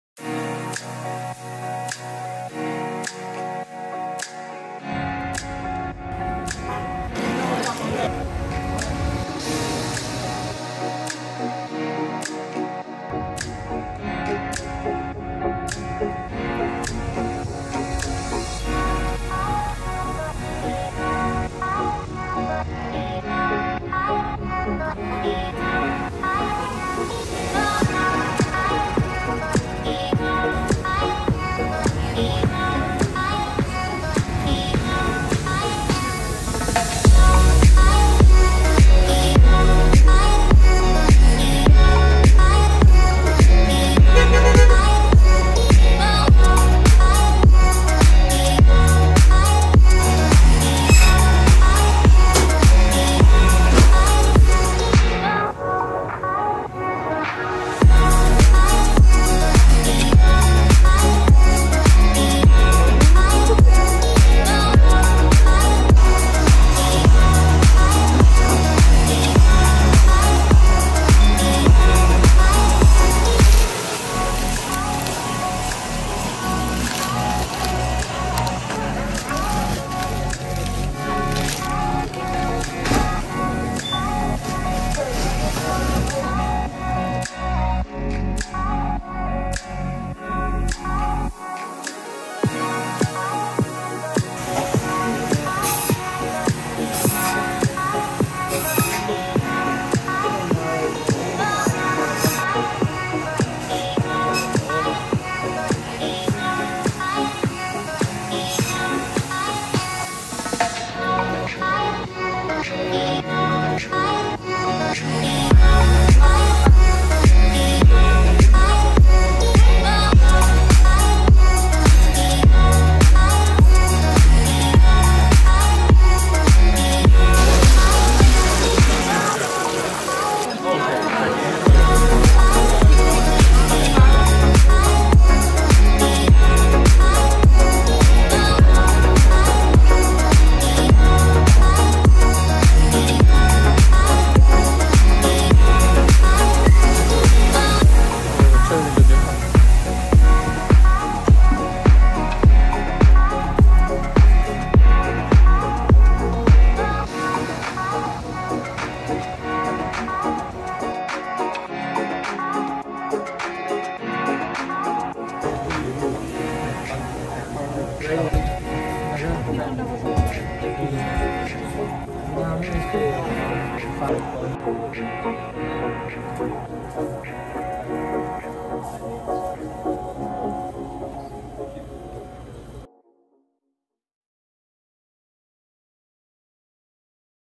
Jackson Heights, NY ||আমেরিকায় বাংলাদেশ। A Piece of Bangladesh in New York, America.
আমেরিকায় বাংলাদেশ। বিদেশে বসে দেশের স্বাদ। চলুন ঘুরে আসি। আশা করি ভালো লাগবে। We are going to Jackson Heights, NY, USA. Dhaka in New York.